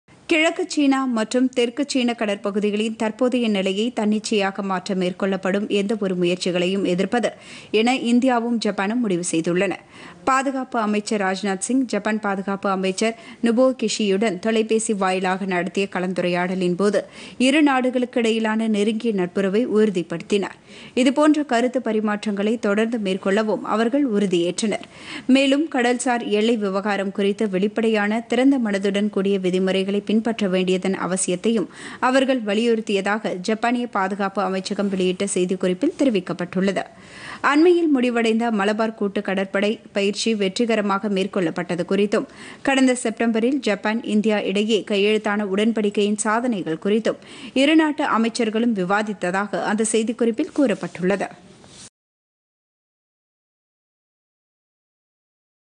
இந்தியா - ஜப்பான் திட்டவட்டம்PodhigaiTamilNews
கிழக்கு சீனா மற்றும் தெற்கு சீன கடற்பகுதிகளின் எல்லையை மாற்றும் முயற்சிகளை   அனுமதிக்க முடியாது -  இந்தியா - ஜப்பான் கூட்டாக அறிவிப்பு.